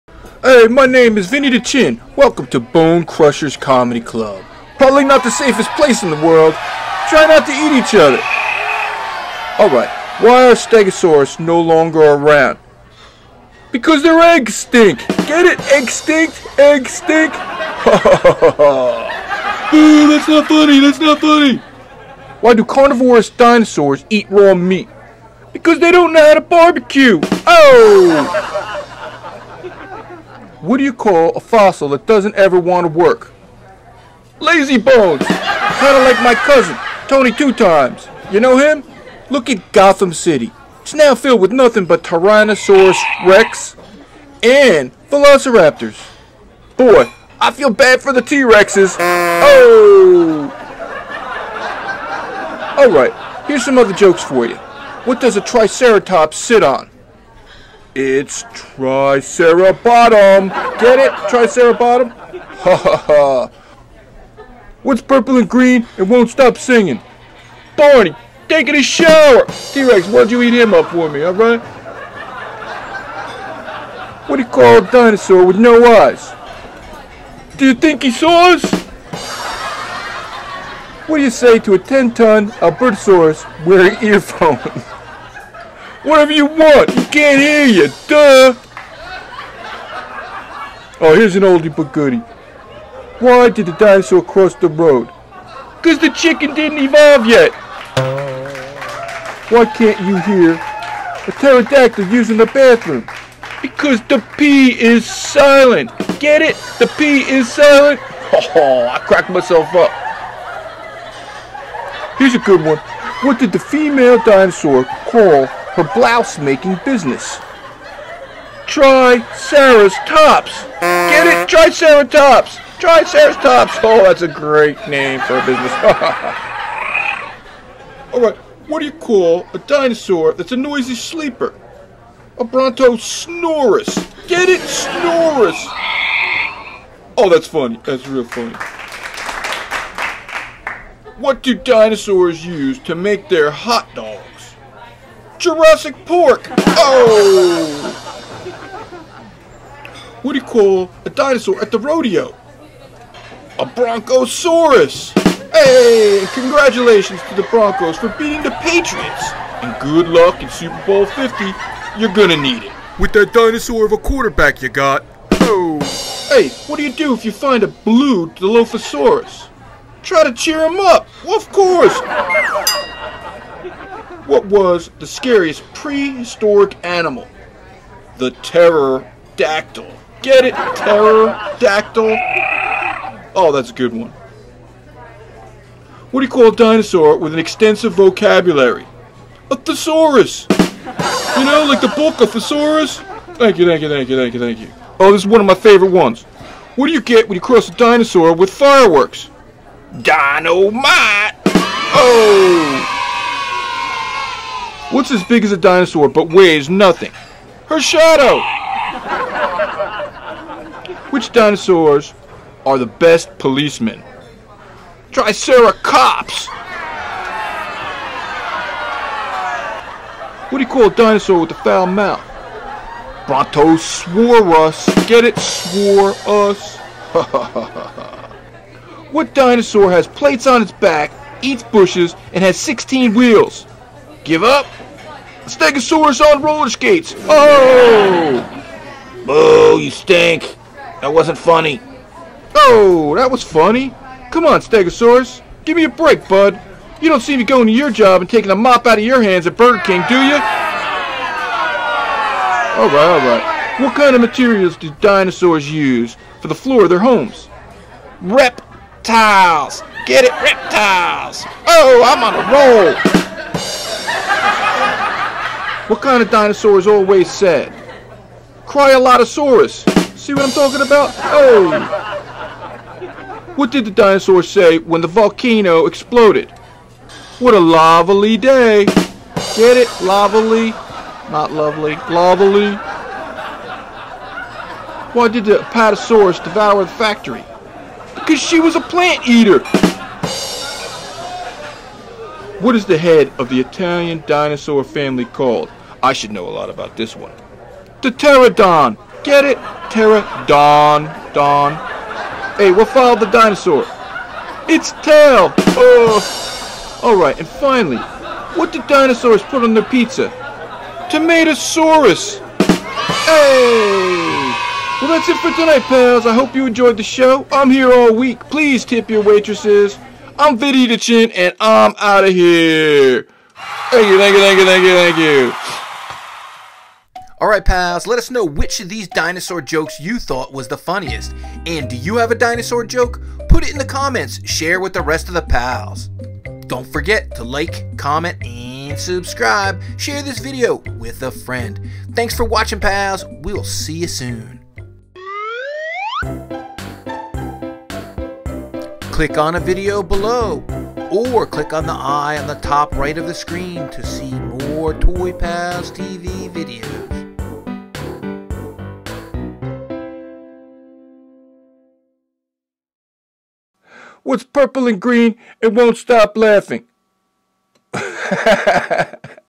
Funny DINOSAUR JOKES for Kids at the Dinosaur Comedy Club
Video with funny clean dinosaur jokes for kids performed by Vinny-da-Chin at Bone Crushers Toy Dinosaur Comedy Club for children.  (You can guess the location).  Vinny tells T Rex Jokes, Stegosaurus Jokes, Velociraptor Jokes and other dino jokes that bring down the house.  Put in the comments your favorite dinosaur joke from the video or put your own that is clean and funny in the comments.  Below are a bunch of the jokes used in the video in case you want to borrow them.

My name is Vinny-da-Chin and welcome to Bone Crushers Comedy Club.  Probably not the safest place on the planet.  I can see we have a rough crowd so please take it easy and try not to eat each other!  Ba-bo-ching

Look at Gotham City, it’s  filled with nothing but Tryannosaurus Rexes and Velociraptors.  Boy I feel bad for the T-Rex’s!

Q: What does Triceratops sit on?
A: Its Tricera-bottom!

Q: Why are Stegosaurus no longer around?
A: Because their eggs stink.

Q: What do you call a fossil that doesn't ever want to work?
A: Lazy bones!  (kinda like my cousin Tony-2-times)

Q: Why do carnivorous dinosaurs eat raw meat?
A: Because they didn't know how to barbecue!

Q: What's purple and green and won't stop singing?
A. Barney taking a shower!

Q: What do you call a dinosaur with no eyes?
A: Doyathinkysaurus! (Do you think he saw us)

Q. What do you say to a 10-ton Albertosaurus wearing earphones?
A. Whatever you want. He can't hear you!

Q: Why did the dinosaur cross the road ?
A: The chicken hadn't evolved yet!

Q: Why can't you hear a pterodactyl using the bathroom?
A: Because the 'p' is silent

Q: What did the female dinosaur call her blouse making business?
A: Try Sara's Tops

Q: What do you call a dinosaur that's a noisy sleeper?
A: A Bronto-snorus.

Q. What did dinosaurs use to make their hot dogs?
A. Jurassic pork!

Q: What do you call a dinosaur at the rodeo?
A: Broncosaurus - Congrats to the Broncos for beating the Patriots and good luck in Super Bowl 50, you’re gonna need it with that dinosaur for a Quarterback you have!

Q. What should you do if you find a blue Dilophosaurus?
A. Try to cheer him up!

Q: What was the scariest prehistoric animal?
A: The Terrordactyl

Q: What do you get if you cross a Triceratops with a kangaroo?
A: A Tricerahops

Q: What do you call a dinosaur with a extensive vocabulary?
A: a thesaurus.

Q: What do you get when you cross a dinosaur with fireworks?
A: DINOMITE!!!

Q: What's as big as a dinosaur but weighs nothing?
A: Her shadow

Q: Which dinosaurs were the best policemen?
A: Triceracops

Q: What do you call a dinosaur with a foul mouth?
A: Bronto-swore-us.

Q: What has plates on its back, eats bushes, and has sixteen wheels?
A: A Stegosaurus on roller skates!

Boo!! you stink. That’s not funny. BOO!..
Hey, stegosaurus, give me a break bud.  You don’t see me going to your job and taking the mop out of your hand a Burger Kind, do you?

What kind of materials do dinosaurs use for the floor of their homes?
A: Rep Tiles.

Q: What dinosaur is always sad?
A: Cryalotosaurus

Q: What did the dinosaur say when he saw the volcano explode?
A: What a lavaly day!

Q: Why did the Apatosaurus devour the factory?
A: Because she was a plant eater!.

What is the head of an Italian dinosaur family called?
A: Ptera Don

Q:What followed the dinosaur?
A: It’s tail!

Q: What do dinosaurs put on their pizza?
A: Tomato-saurus

Well that’s it for tonight pals, I hope you enjoyed the show.. I’m here all week.  Please tip your waitresses.
I’m Vinny-da-chin - out.

Thanks PALS!!